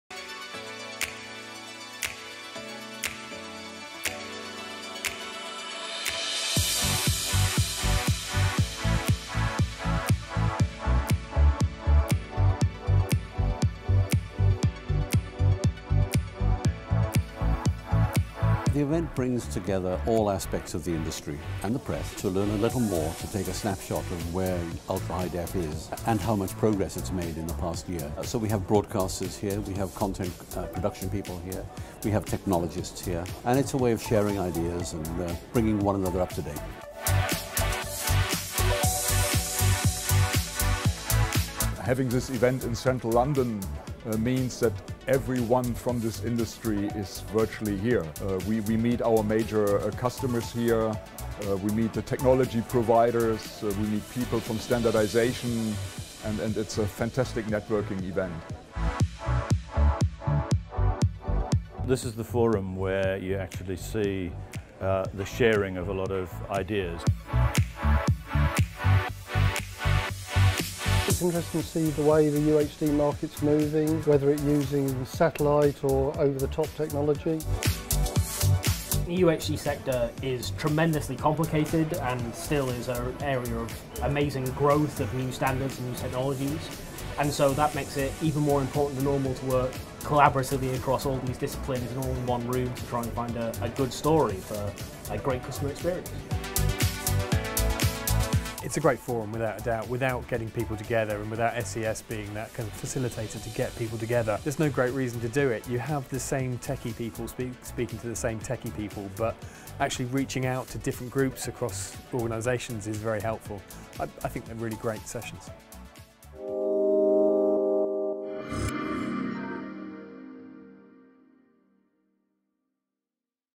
SES Ultra HD Conference 2017
SES Video hosted its fourth Ultra HD Conference in London on 20 June 2017.